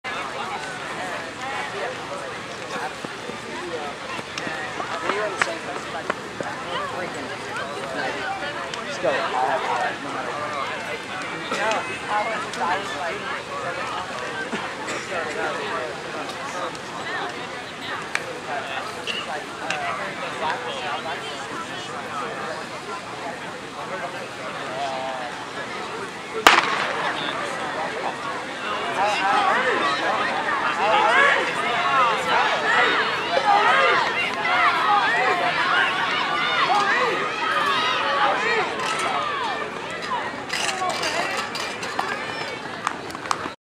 Campbell County Track from TheVolunteerTImes.com: Girls 100 meter hurdles.
Campbell County's Kristin Chapman in the 100 meter hurdles at Oak Ridge 5-7-11.